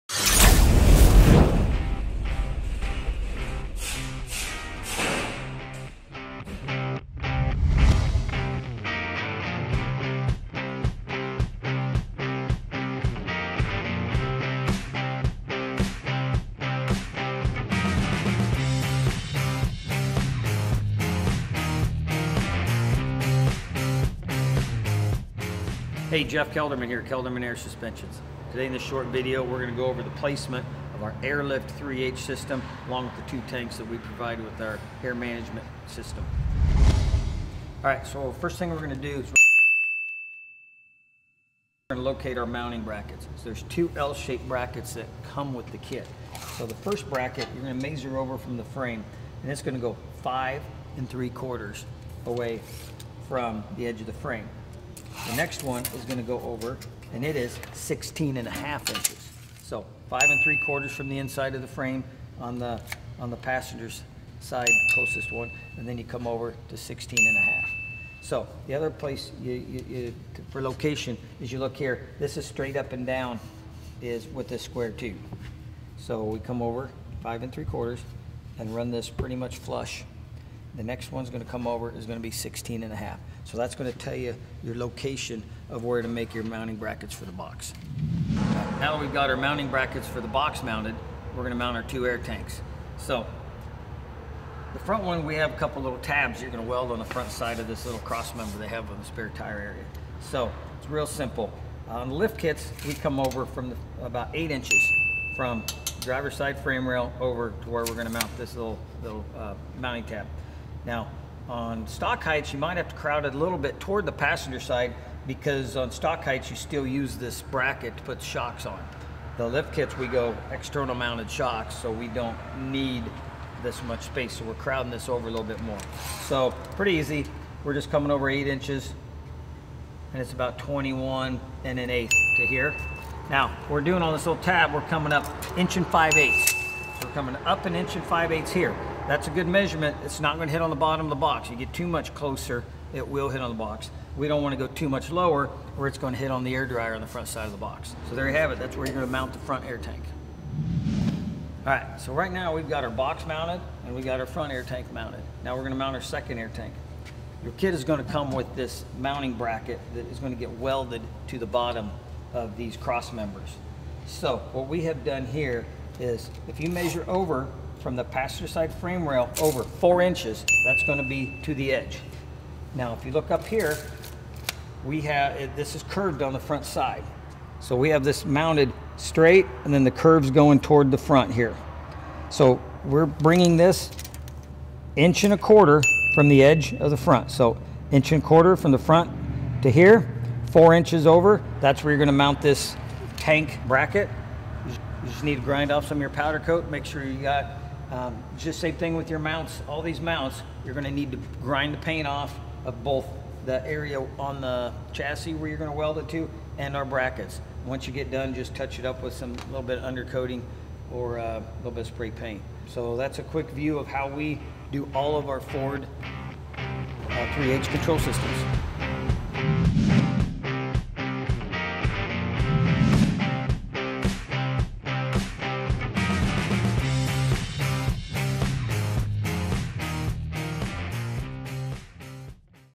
Kelderman Air Suspension - Air Lift 3H Controls mounting tutorial
This video shows suggested mounting and install tips for the Air Lift 3H Air Control System available from Kelderman for use with our air suspension systems.